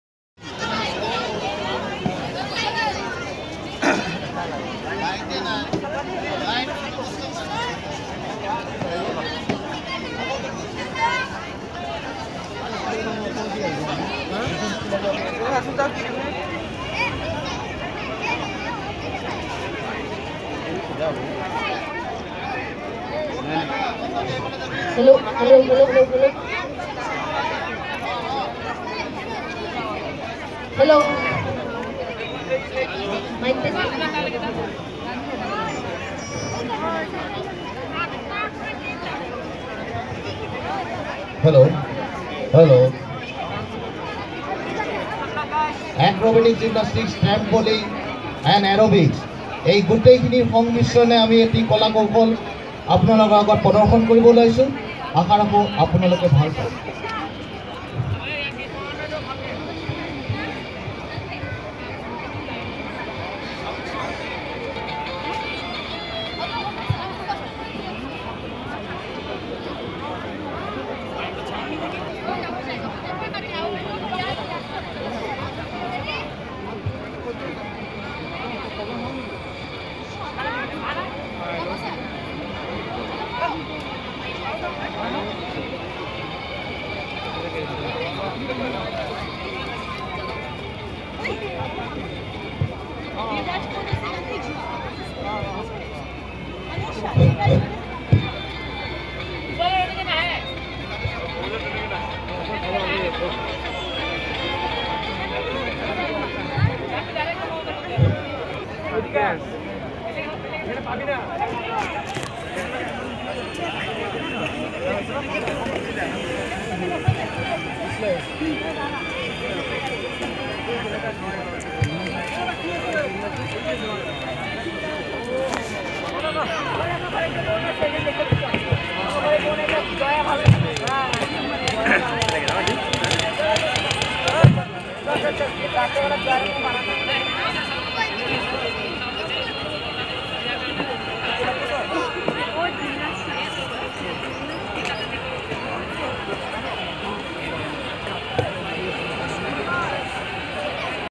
Gymnastics | At Namami Brahmaputra VDO 2
Gymnastics Performance at Namami Brahmaputra River Festival